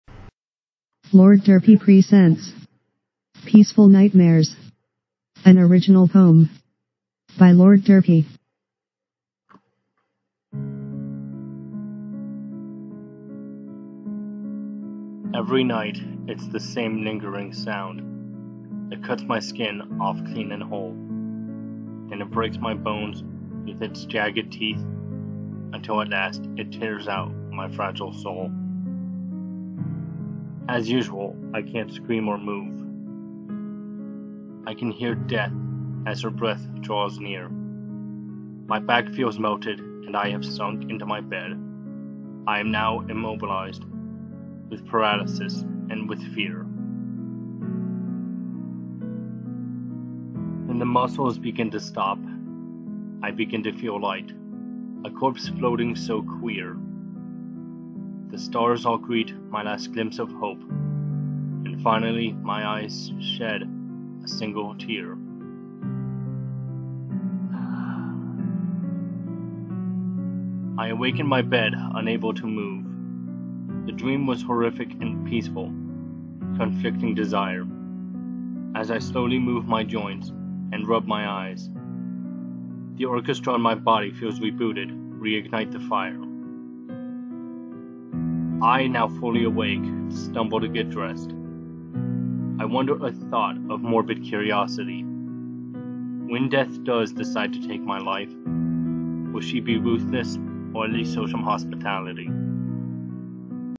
Peaceful Nightmares: An Original Poem
Well, MetaGAL is still acting up, and I need to replay Wade Hixton. I need a video...How about you guys enjoy one of my poems? I wrote this after having one whole week of nothing but weird dreams that were horrific yet oddly soothing.

Stay Awesome! /)^3^(\